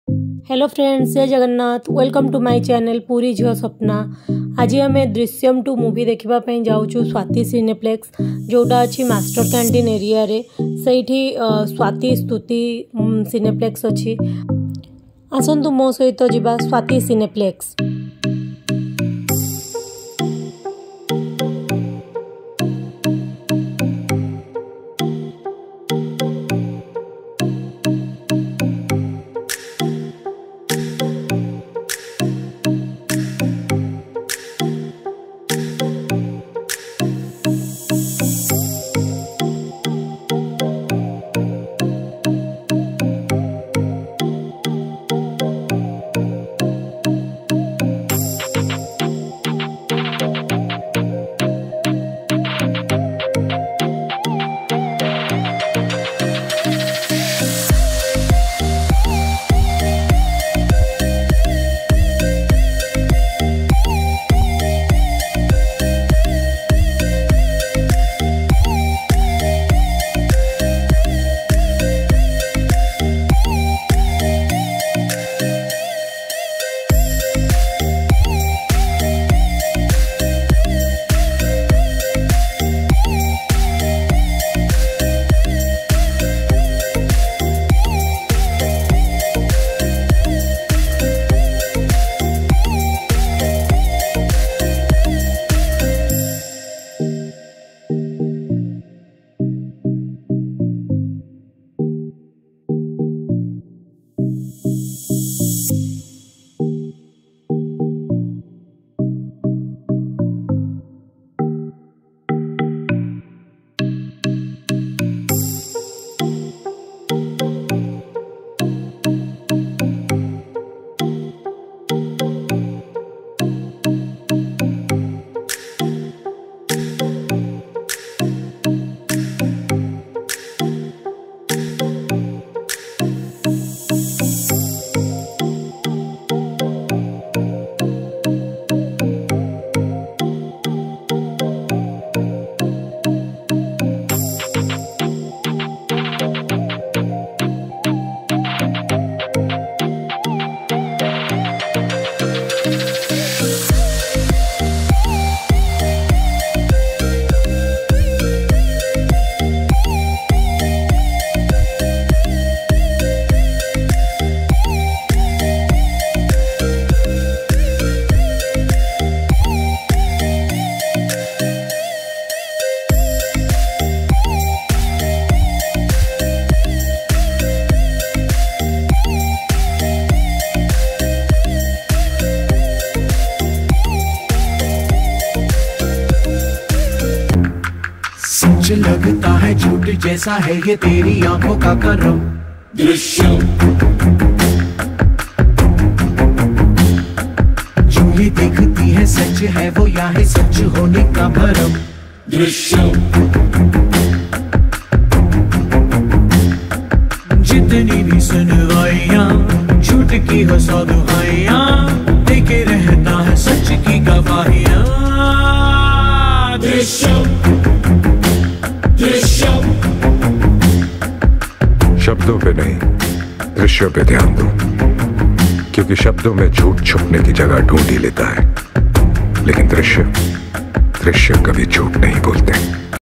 ଆସନ୍ତୁ ଯିବା SWATI CINEPLEX | DRISHYAM 2 | Movie Review | Bhubaneswar | Puri jhia sapna
ଆସନ୍ତୁ ଯିବା SWATI CINEPLEX | DRISHYAM 2 | Bhubaneswar | Puri jhia sapna |

Swati Cineplex (CHRISTIE 4K DOLBY ATOMS)
Location 📍 : Swati Talkies

Booking Mode : Online / Offline

Ambience : Good
Seat : Luxurious Recliner / Sofa Slider / Diamond / Gold
Parking : Available

Interior : 4/5
Cleanness : 5/5
Staff Behavior : 5/5
Luxurious Recliner : 5/5
Screen Size : 4/5
Sound : 4/5
Screen Clarity : 4/5

Food Court :
Price : Moderate
Payment Mode : QR Based / Cash
Service : 4/5
Food Quality : 4/5

Overall Experience :
Swati Complex : 9/10
Drishyam Movie : 10/10
Preferred Group : Family / Couple / Single